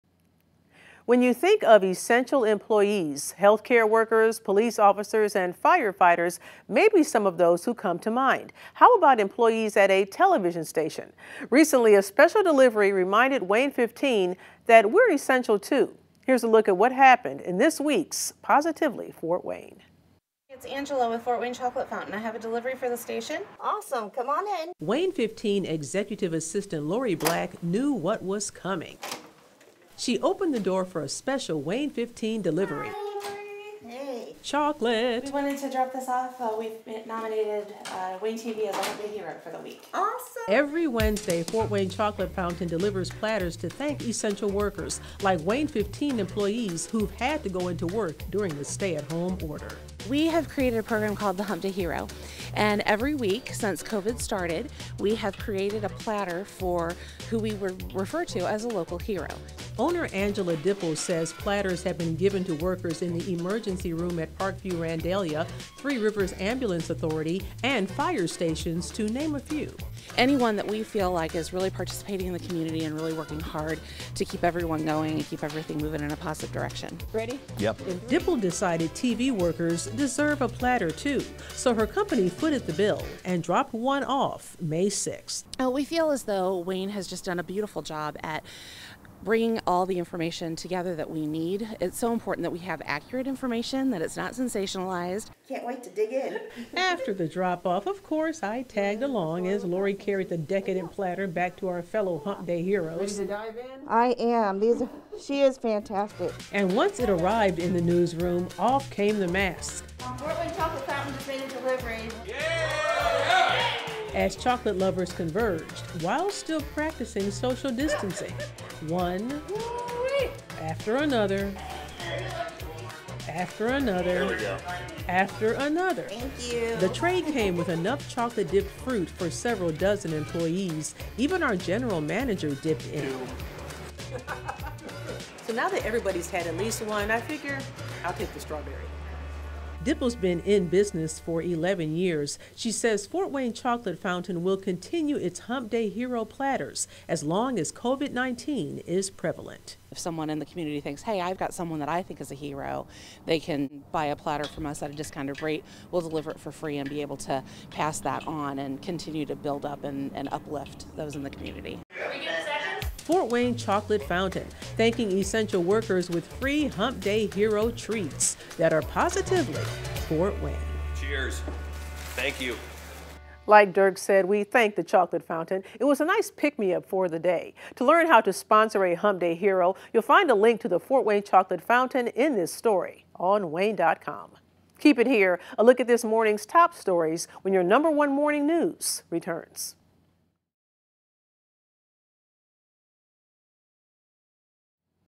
Hump Day Hero deliveries are Positively Fort Wayne
Fort Wayne Chocolate Fountain delivers free platters to essential workers.